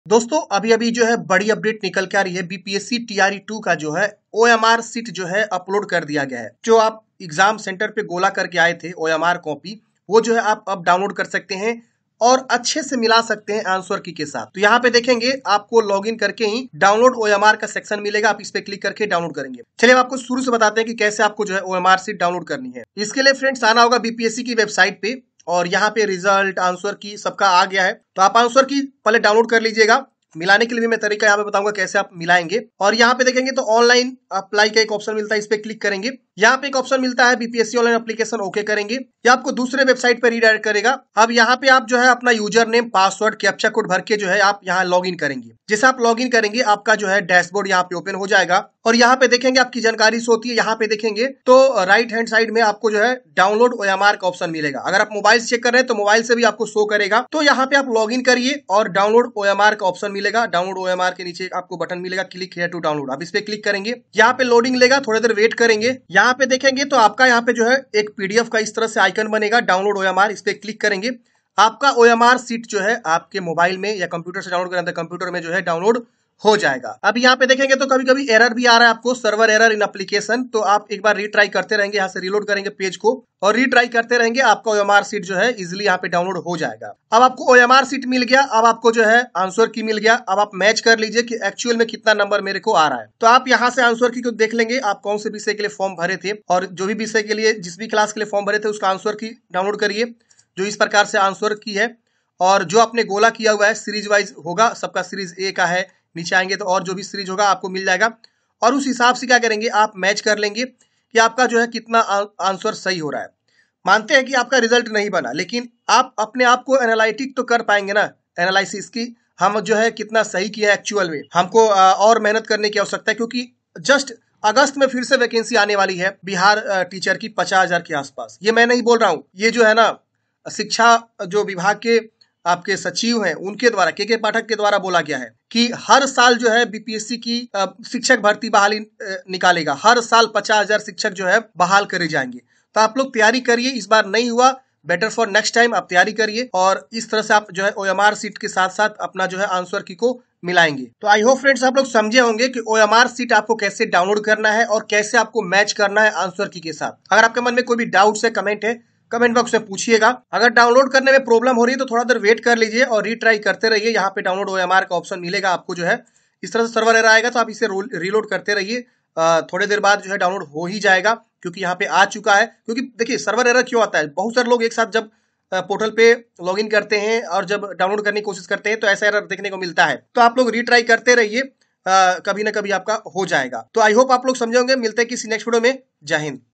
BPSC TRE 2 OMR Sheet Download Kaise Kare | How to download BPSC TRE 2 Exam OMR Sheet
Direct Link To Download BPSC TRE 2 OMR Sheet